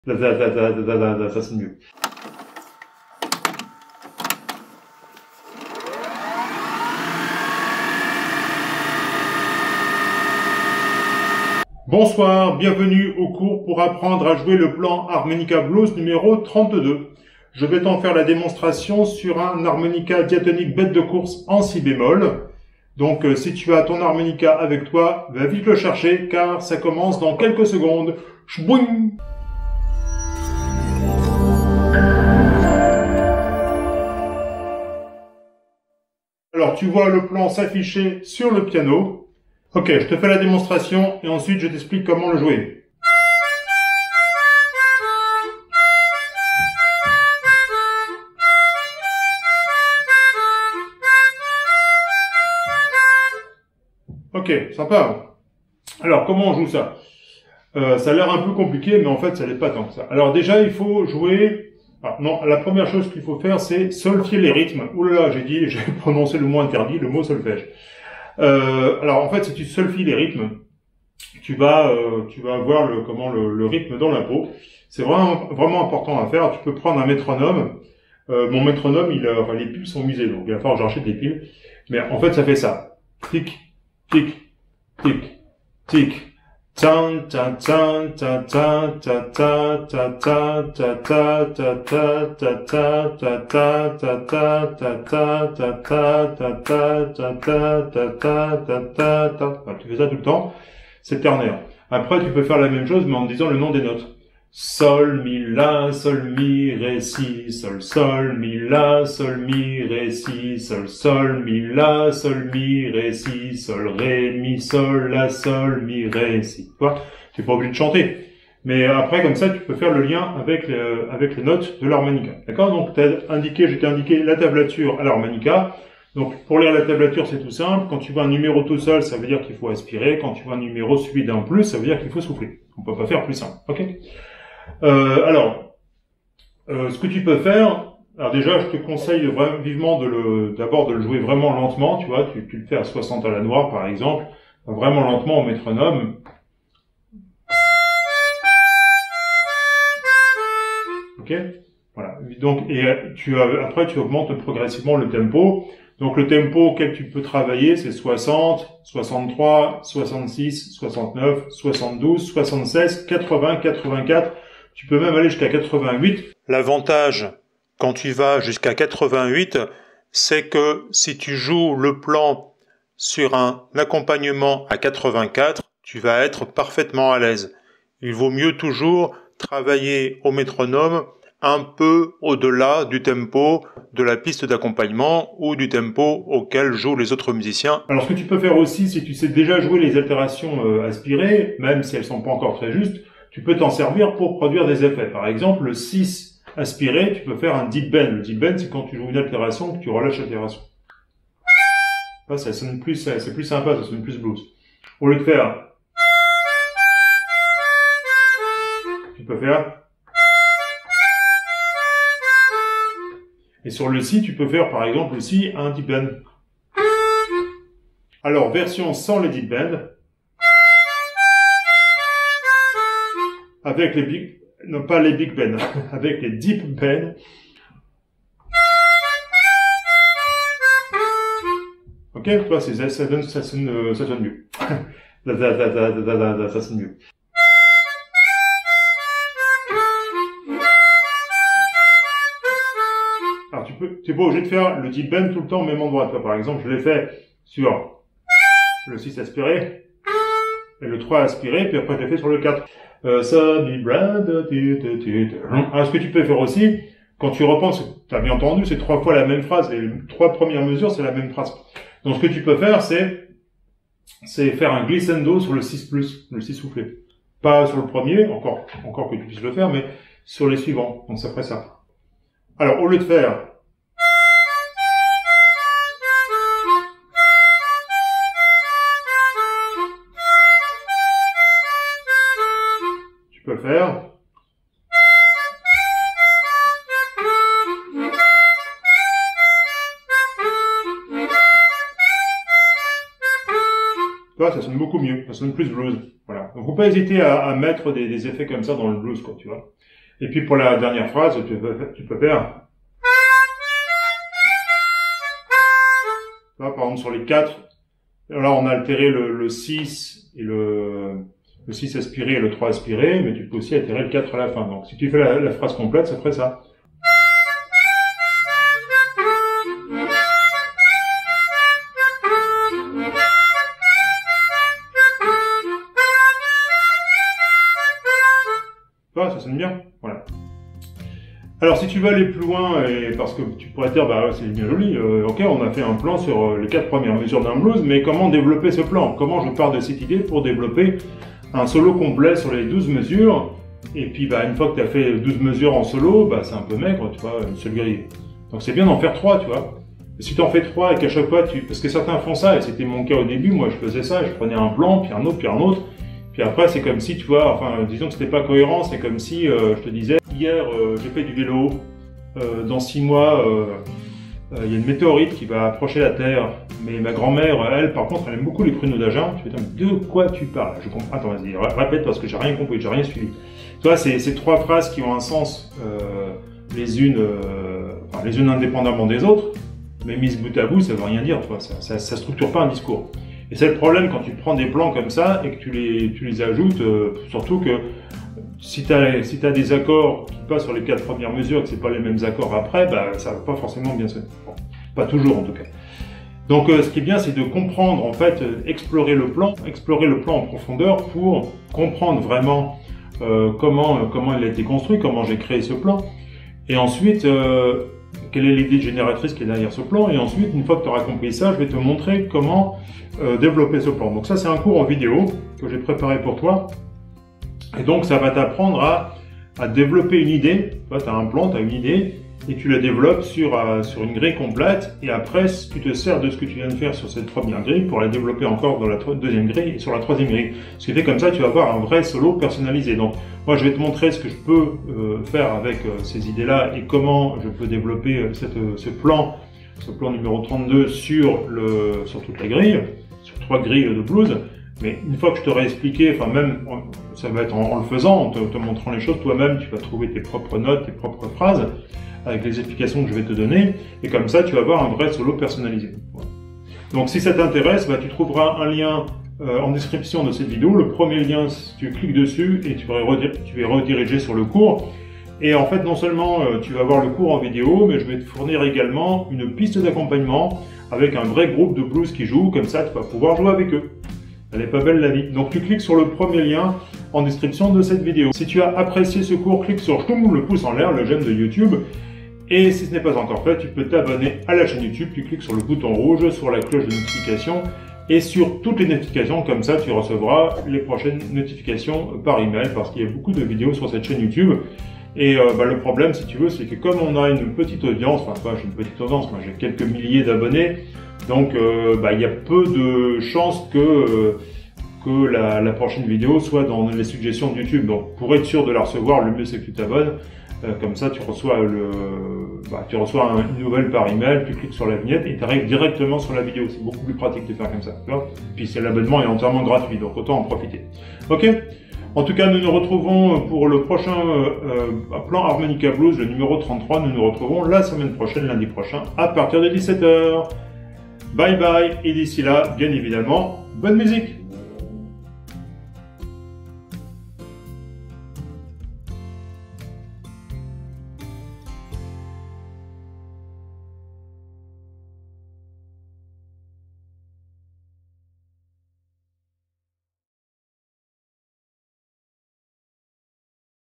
Plan Harmonica Blues 032 - Apprends à jouer de l'harmonica blues - aucune altération - Cours OFFERT
Dans ce cours en accès libre, tu vas apprendre à jouer un super plan de blues à l'harmonica.
On va faire chauffer l'harmo ! 🔥
Que des notes naturelles : aucune altération n'est requise, pour jouer ce super plan en 2e position.